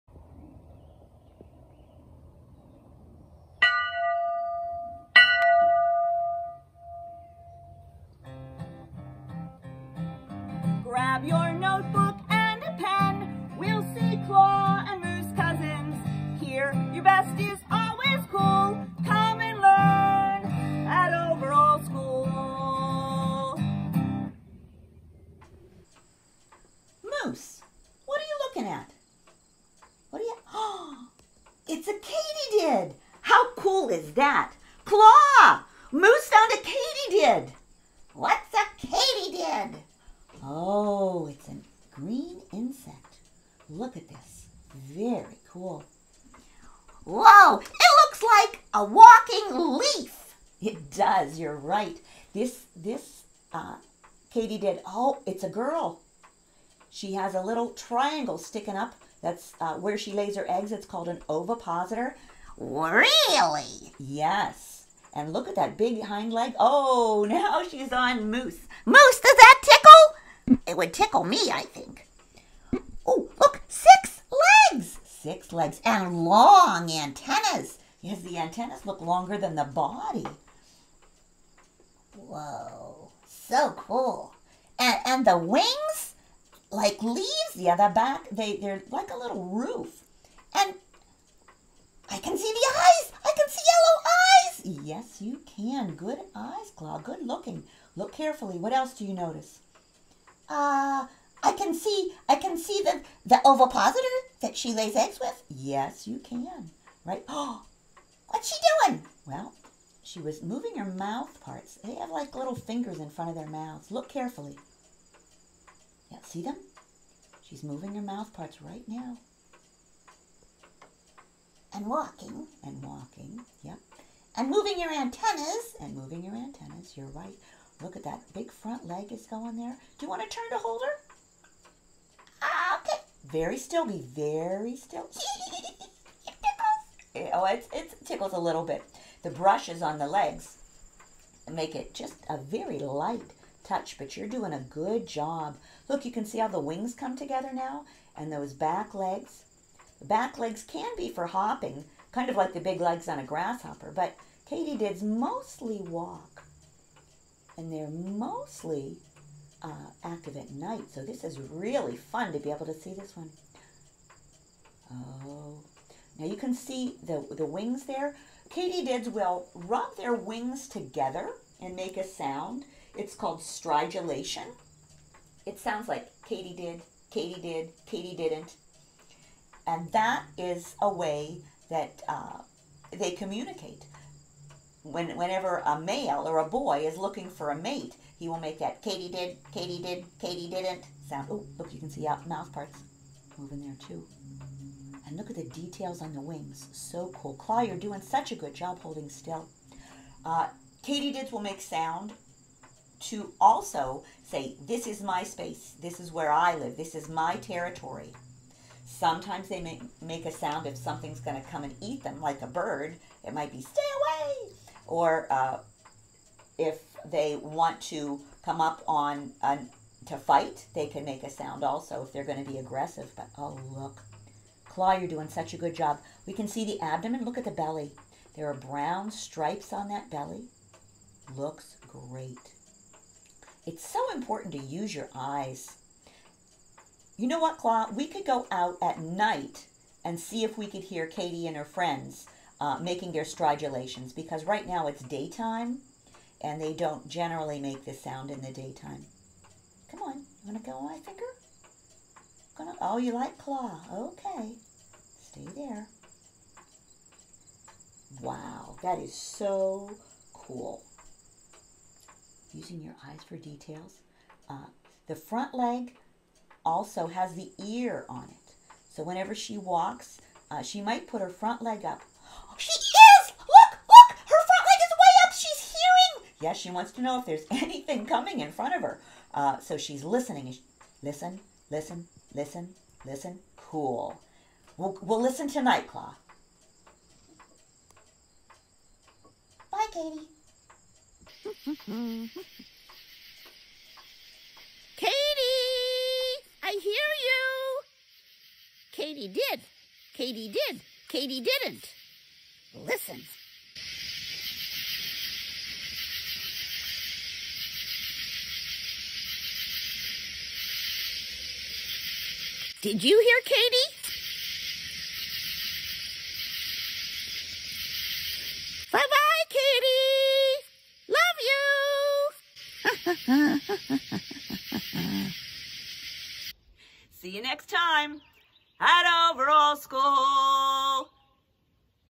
Overall School: Science, Katydid!
Claw and Mrs. Overall discuss the katydid that Moose found.
Topics discussed include:
- insects parts
- sex
- sound